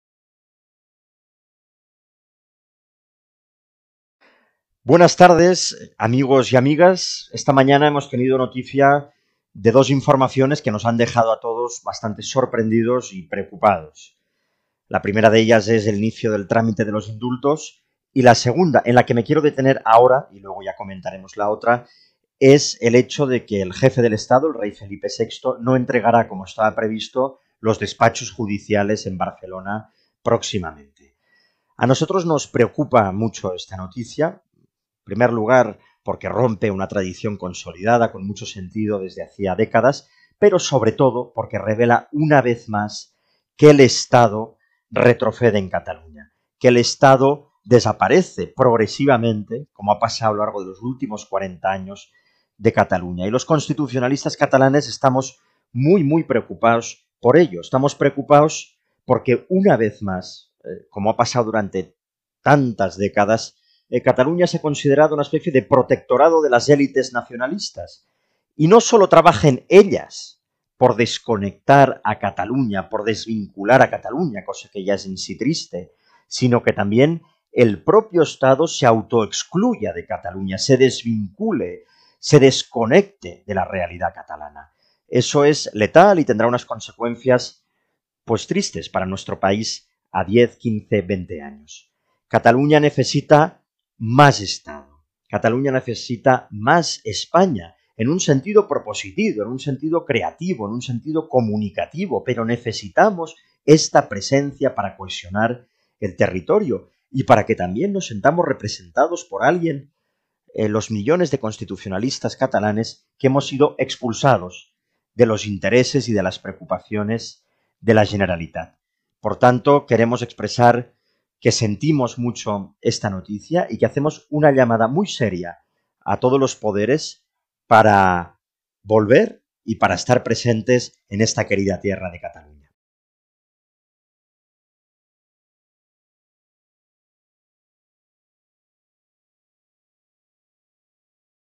Societat Civil Catalana lamenta que el Jefe del Estado no pueda venir a Barcelona
Societat Civil Catalana lamenta que el Jefe del Estado no pueda venir a Barcelona, rechaza convertir Cataluña en un protectorado de las élites nacionalistas y llama a recuperar de forma clara la presencia del Estado en Cataluña para evitar una desconexión irreversible.